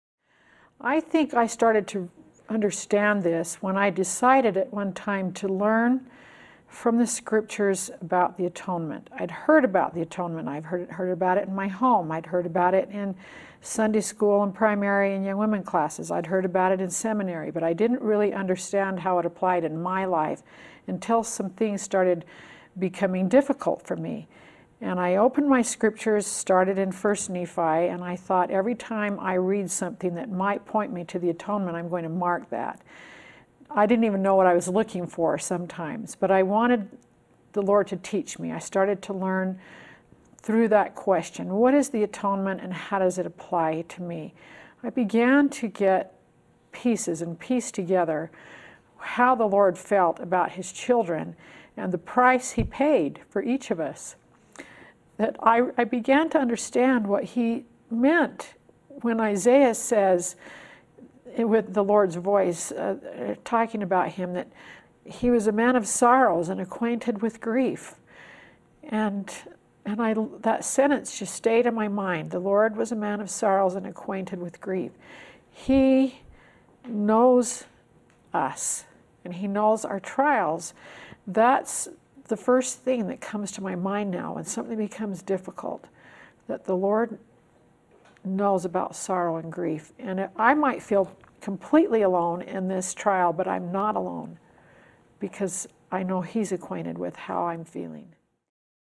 Mormon Women Atonement Brings Comfort
How can we know that during these times we are not abandoned or forsaken, but that Heavenly Father is there? Join us as Mormons Sister Julie B. Beck answers your questions about Trials.